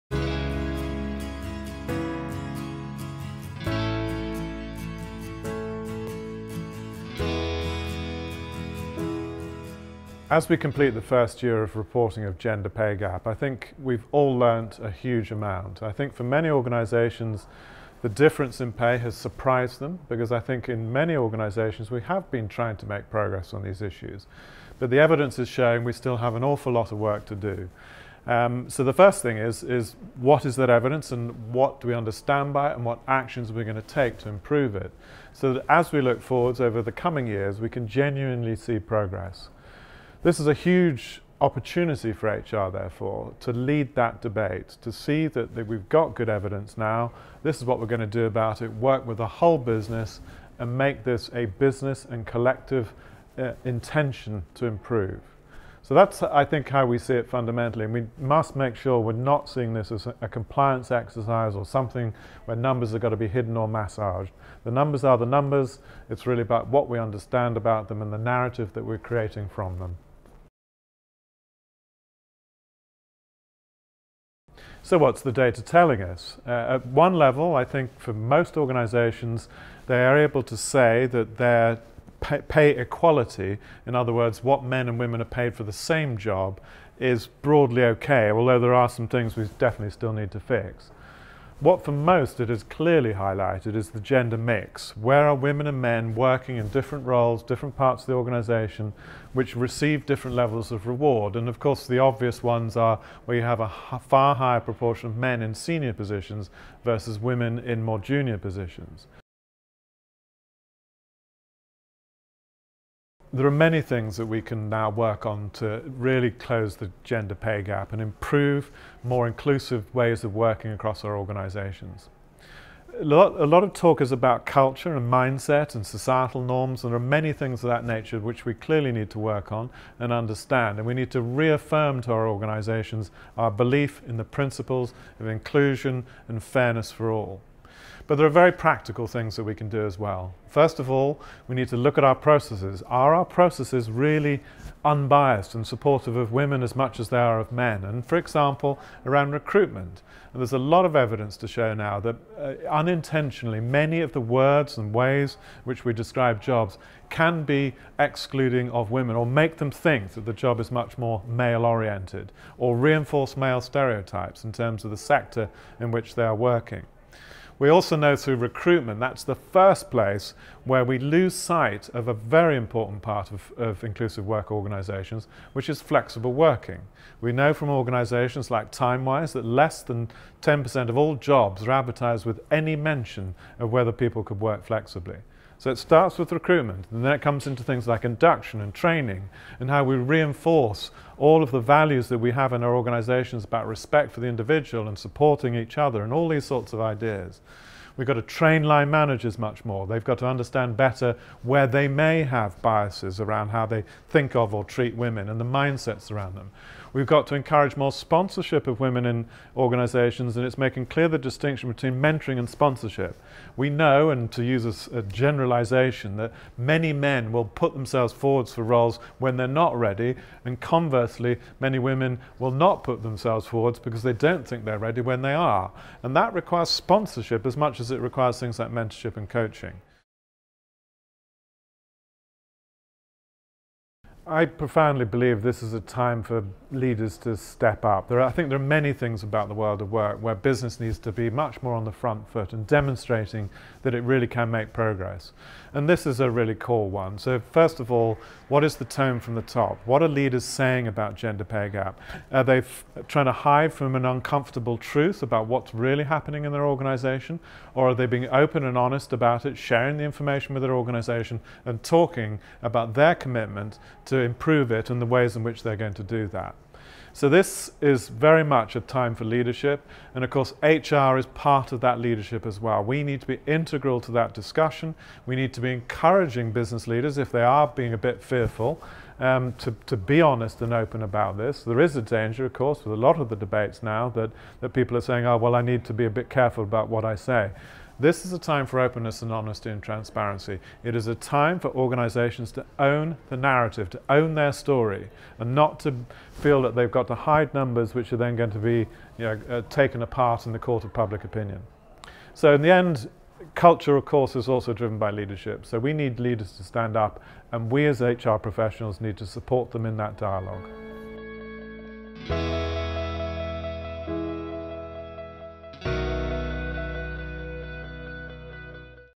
Gender Pay Gap Reporting
From 6 April 2017 employers in Great Britain with more than 250 staff are required by law to publish their gender pay gap information, covering pay and bonuses. With the deadline fast approaching, Peter Cheese, CEO at CIPD shares his reflections and thoughts on gender pay pap reporting and the need to create better workplaces.